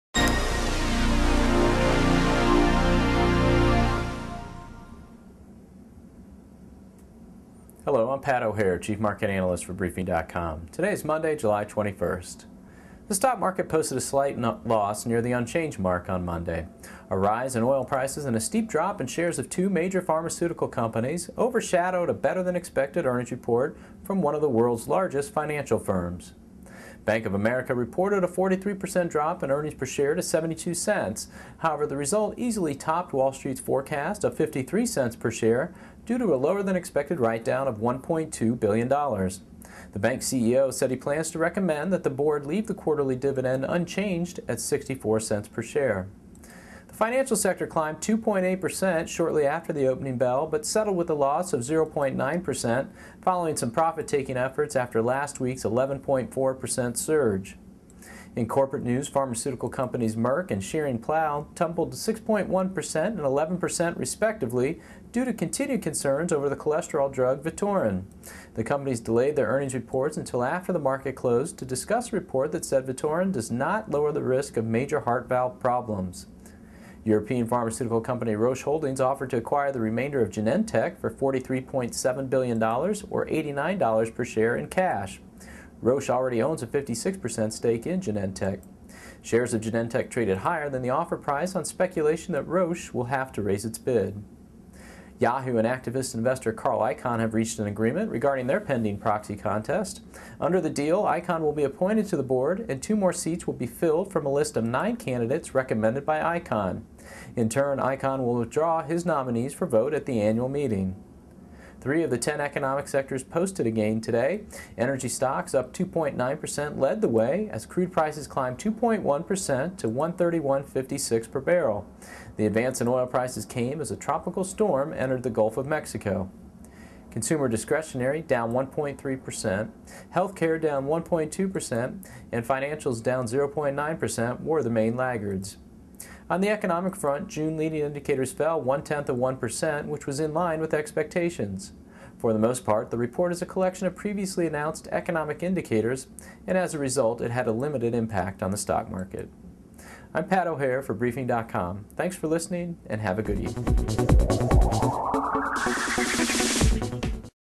Daily Market Wrap for July 21, 2008
Stocks end nearly unchanged; crude climbs.